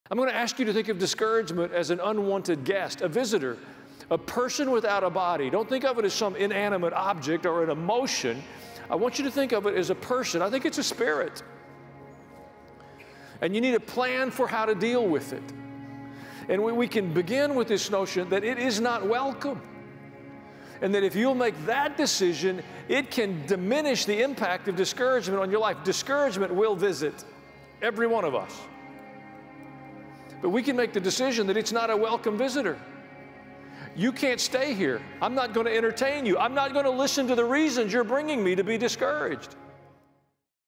Diminish the impact of discouragement
➡️  We need a plan for discouragement. We have to learn how to encourage and help one another, and to encourage ourselves in the Lord. We have to learn to guard our hearts.

🔴  Chapters:
0:00 -